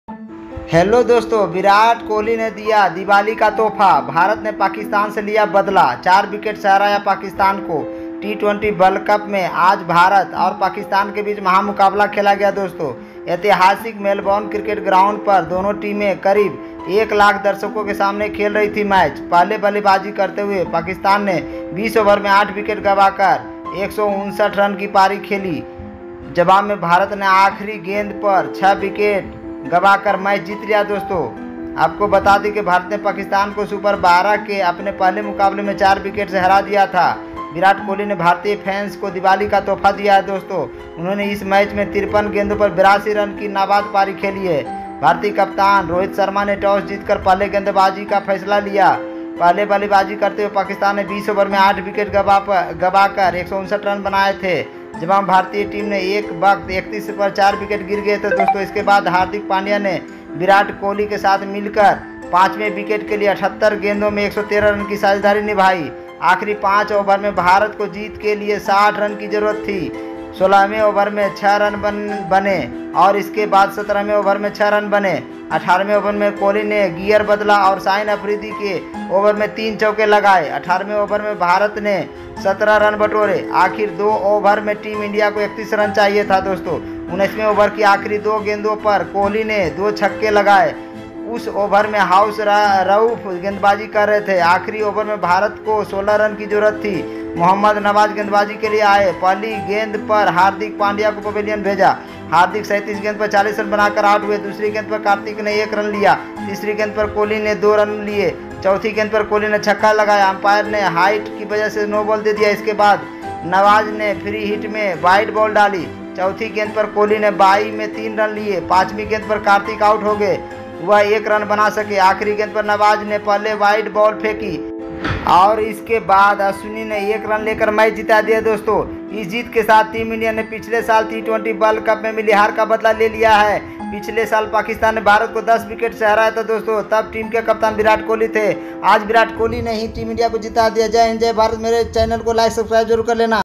भारत से टकराओगे चुर चुर हो जाओगे ,ind vs pak t20 world cup highlights ,india win by 4 wickets
photo credit- bcci ,icc and respected owners